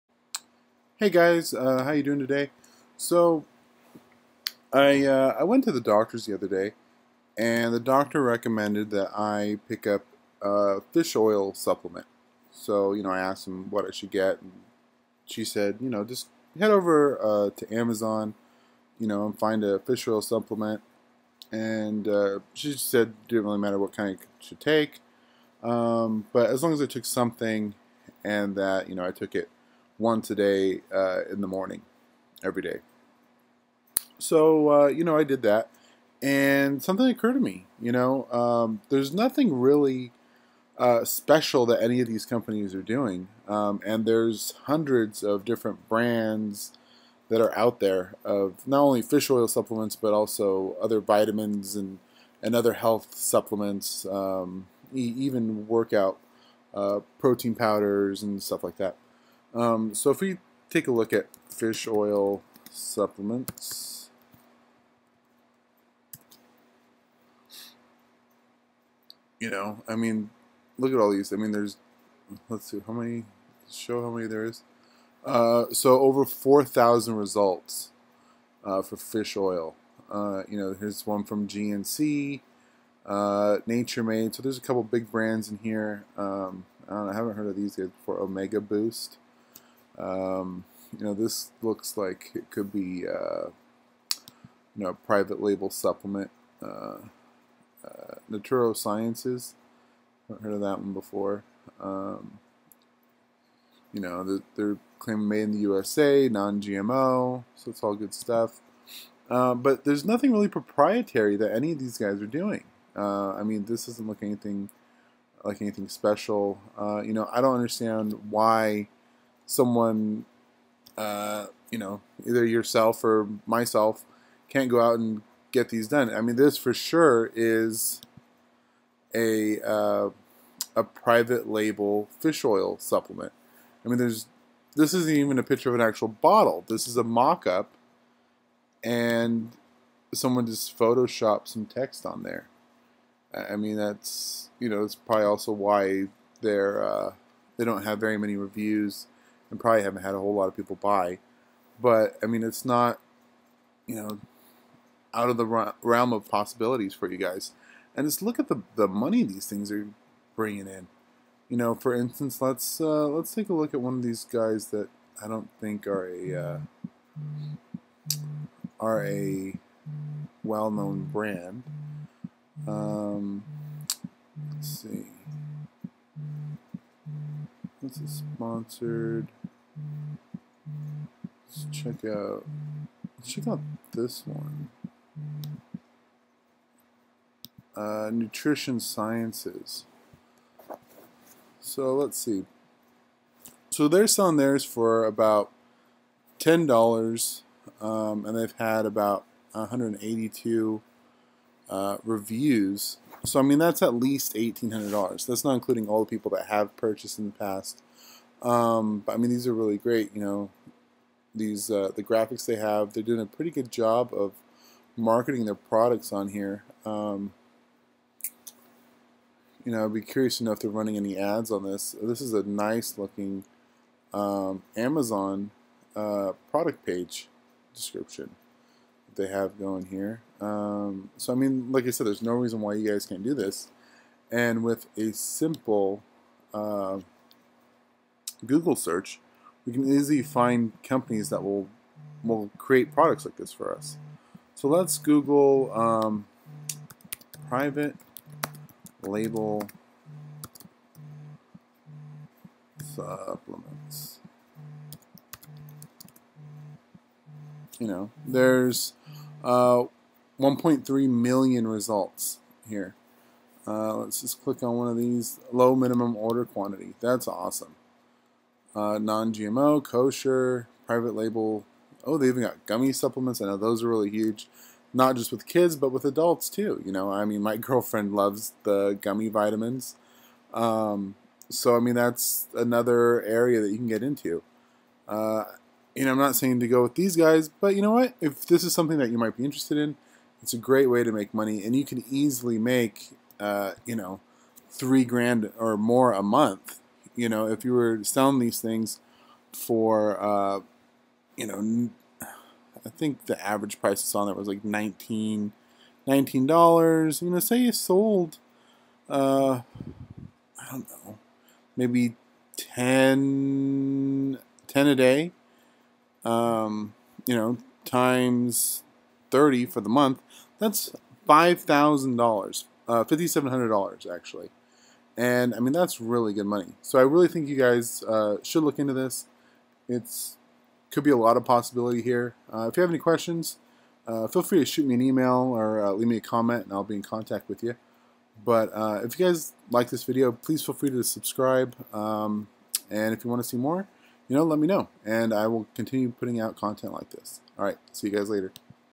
Make $5,700 a Month with Private Label Supplements | Private Label Vitamins
Learn to make $5,700 a month with private label supplements on Amazon and your your personal website.

In this video I will go over how you can start making money in the supplements industry and eventually grow your business to $5,700 plus every month.

You can find a supplier, research their capabilities and if they have a product you want you brand under your own unique supplements company name and you are on your way. You can then send your inventory over to Amazon FBA and have an almost fully automated business.